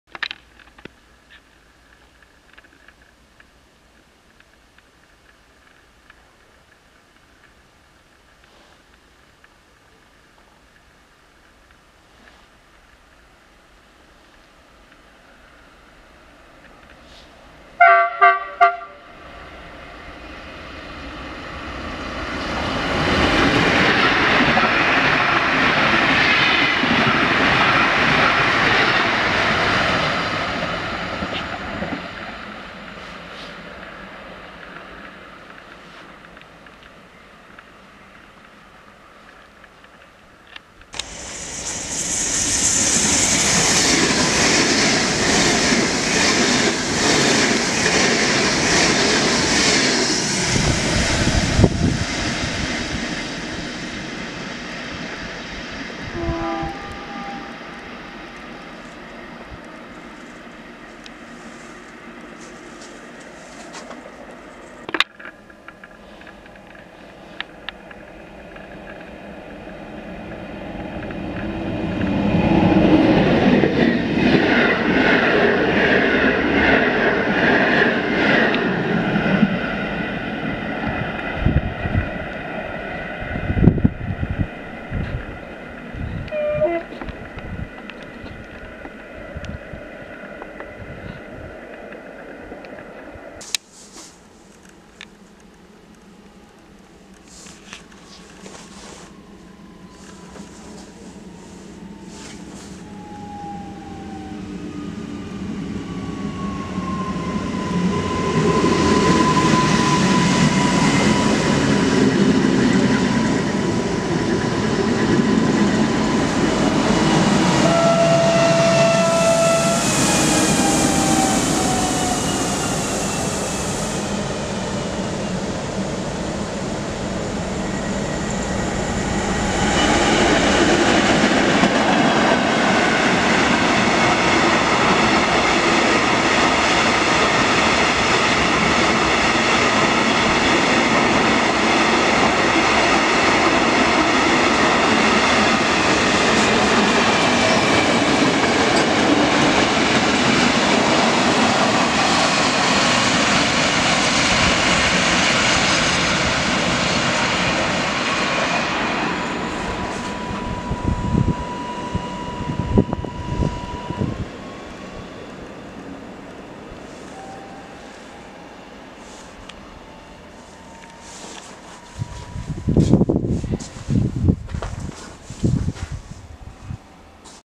Zadymkowo wieczorny klimat PKP Dziadówki
Nagrane składy
1-IC "Żeromski"
2-EIP
3-TLK Malinowski
4-Regio w mijance ze składem roboczym